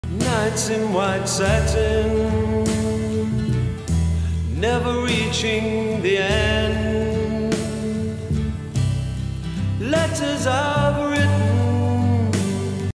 Jellyfish 🪼 in white satin
Lots of medusas swimming around. They look like wearing a white satin dress